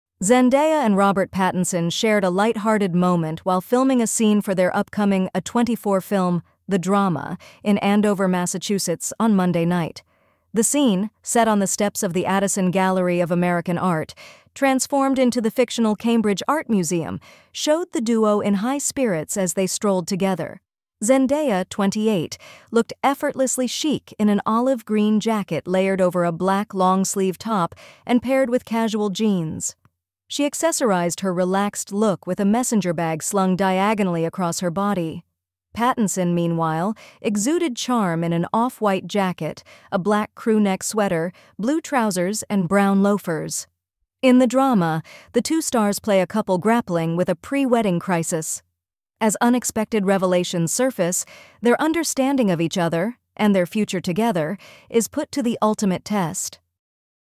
Zendaya & Robert Pattinson Shine on Set of A24’s The Drama in Andover, MA! 🎬 Behind-the-Scenes Scoop
Zendaya and Robert Pattinson light up the set of their upcoming A24 film, The Drama, with a charming moment in Andover, MA! 🎥 The stars were seen filming a pivotal scene on the steps of the fictional Cambridge Art Museum. Zendaya stuns in a casual olive jacket and jeans, while Robert keeps it cool in an off-white jacket and loafers. The film follows a couple facing shocking revelations just days before their wedding. Don’t miss this glimpse behind the scenes of what’s shaping up to be an unforgettable movie! 🌟"